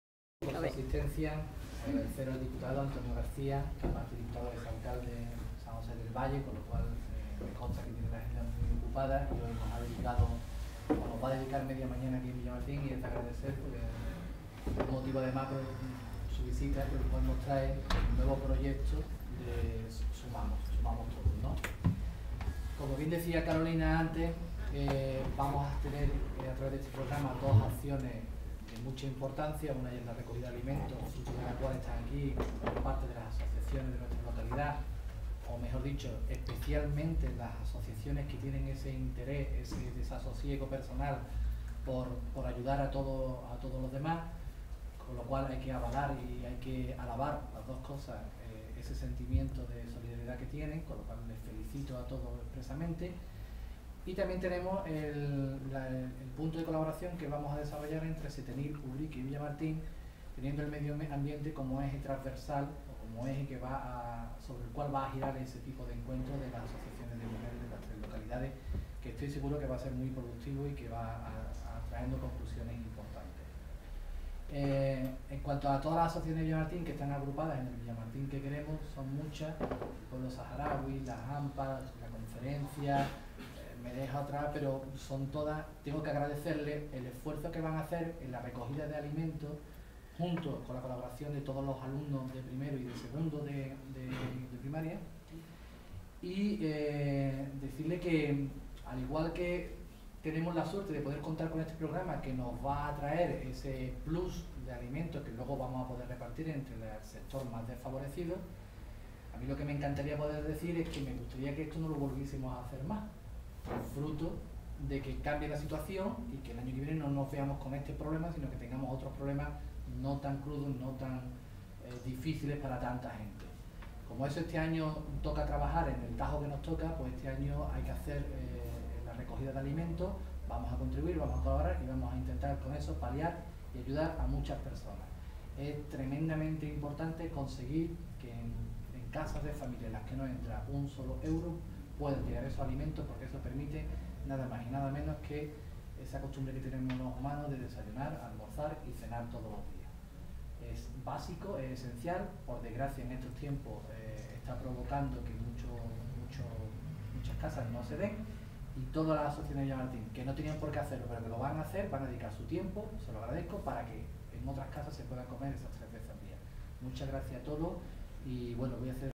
EL ALCALDE DE VILLAMARTÍN SOBRE PROGRAMA SUMAMOS
El alcalde de Villamartín, Juan Luis Morales, habla durante el acto de presentación del programa "Sumamos". En la localidad, dicho proyecto contempla actividades dirigidas al alumnado de infantil y primaria con el objetivo de dar a conocer a las ONGS que trabajan en el municipio, así como concienciar a los escolares sobre la importancia de los valores de solidaridad para conseguir un mundo mejor. También contempla actividades dirigidas a la ciudadanía en general organizadas por el grupo ciudadano "El Villamartín que Queremos" con una recogida solidaria de alimentos.